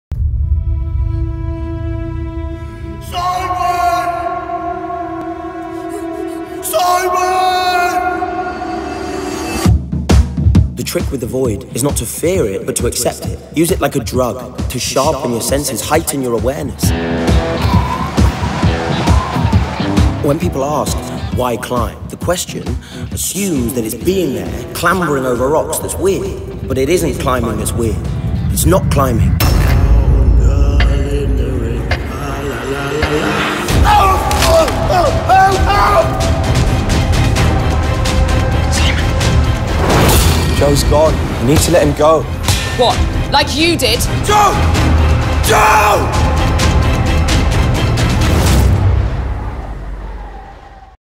TOUCHING THE VOID | TRAILER
Following critically acclaimed runs at the Bristol Old Vic, Royal & Derngate, Northampton, Royal Lyceum Theatre Edinburgh, Hong Kong Festival and on Tour in the UK Tom Morris’ production of Touching the Void will see original cast members Fiona Hampton, Patrick McNamee and Josh Williams return to the show, joined by Angus Yellowlees.